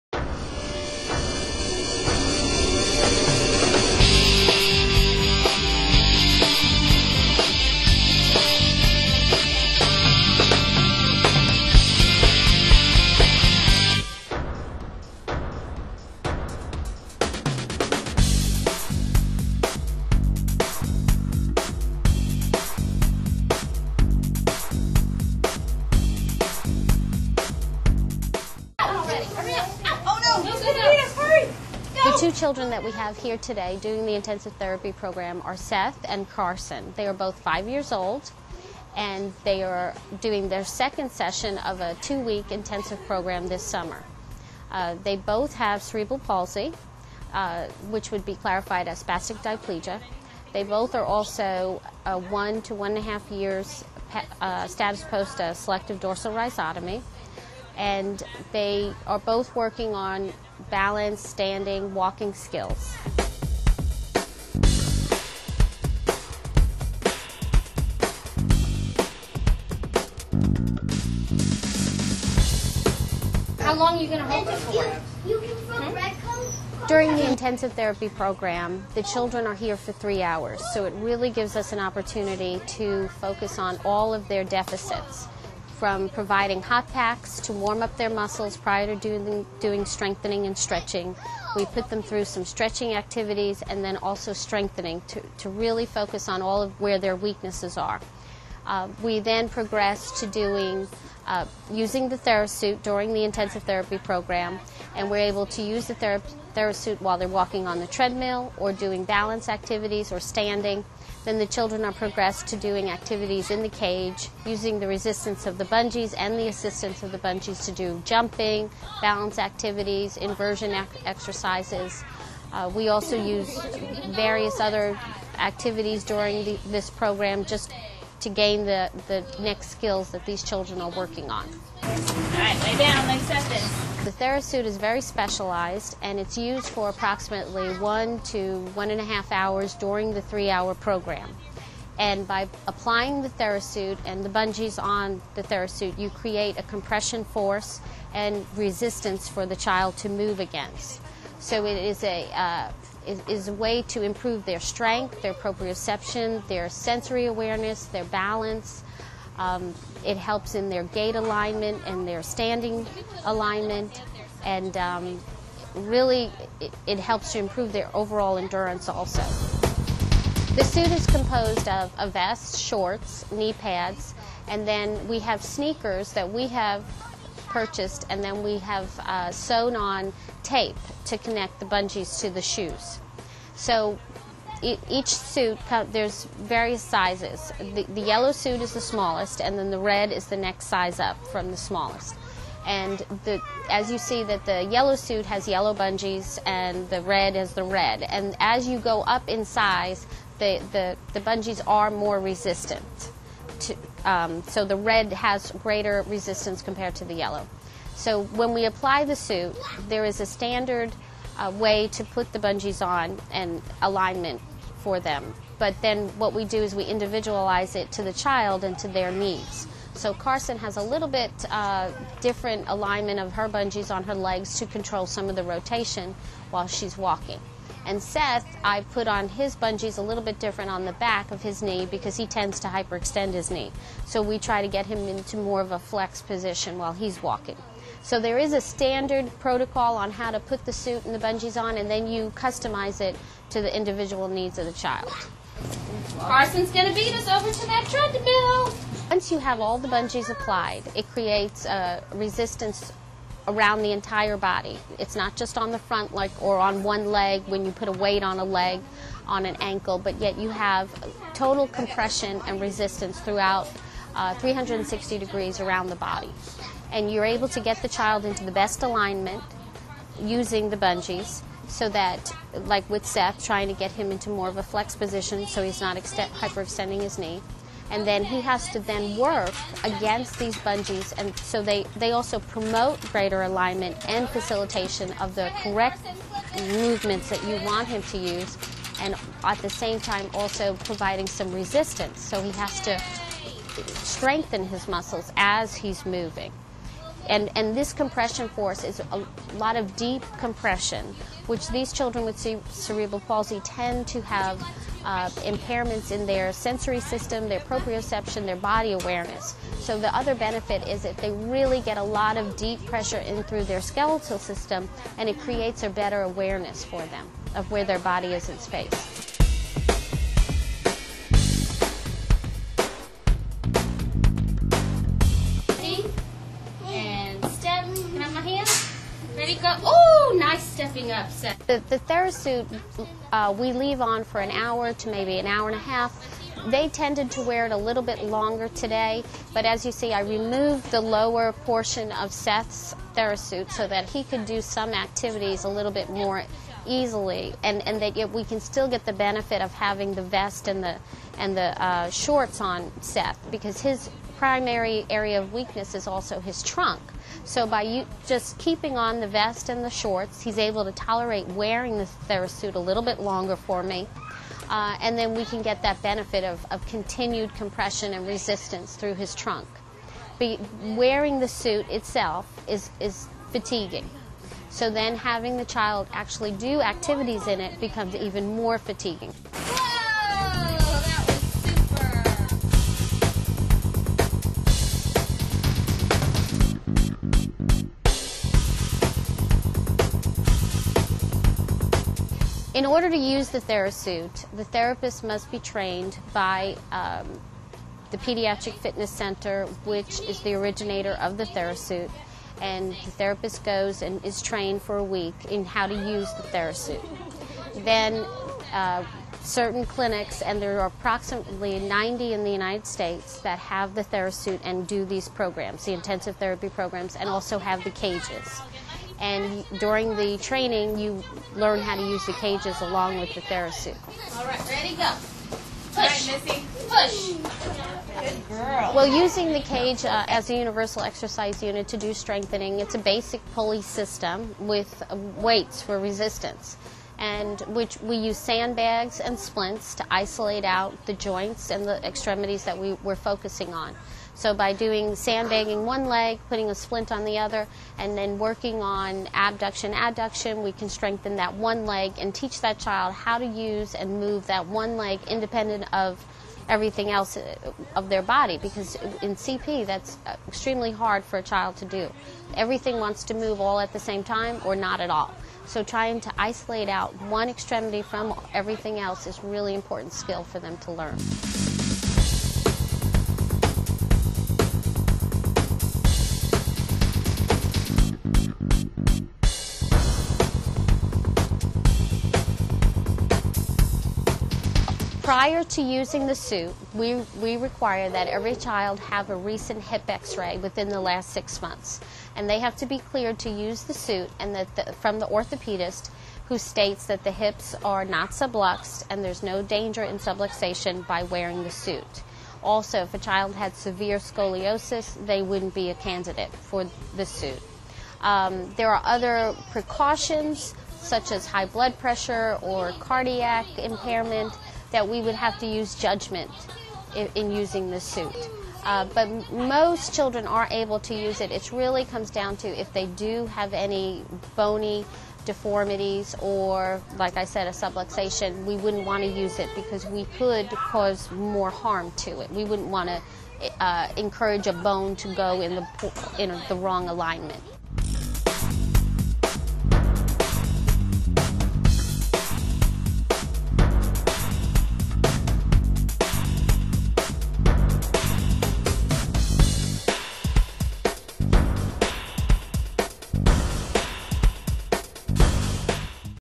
The TheraSuit Method for children with Cerebral Palsy
This video series takes a look at Intensive Therapy Programs and their benefits for children with CP.  In this segment, Pediatric Physical Therapist, Noreen Scott, gives an exclusive interview on how the TheraSuit is used and the areas it works.

We also get a chance to observe two children who use the TheraSuit in their individual therapy programs.

Noreen Scott is based in Dacula, Georgia.  She owns Tender Ones Therapy Services which is one of only around 90 clinics in the US trained in the TheraSuit Method.